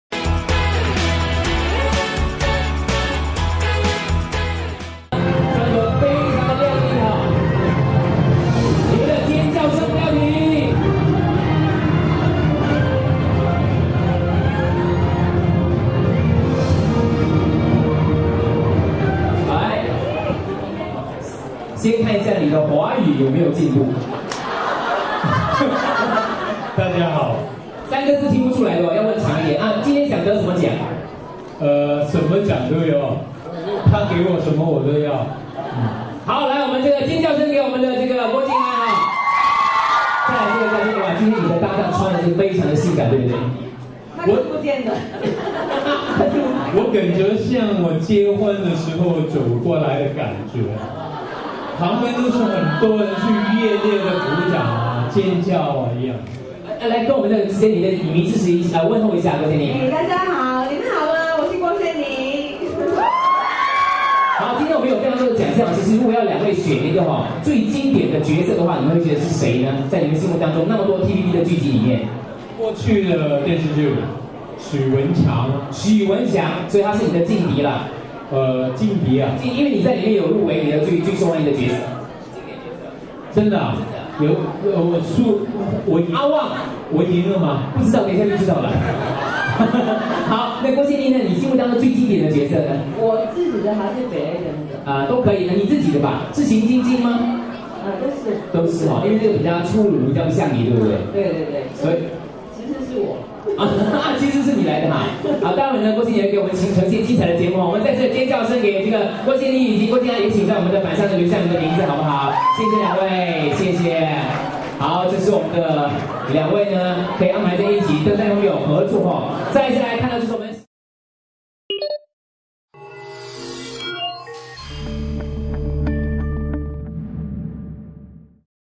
Hotties Roger & Sonija bewitch (Starhub TVB Awards Ep 3.5 )
TVB artistes Roger Kwok Chun On and Sonija Kwok ooze charm and elegance at the Starhub TVB Awards red carpet.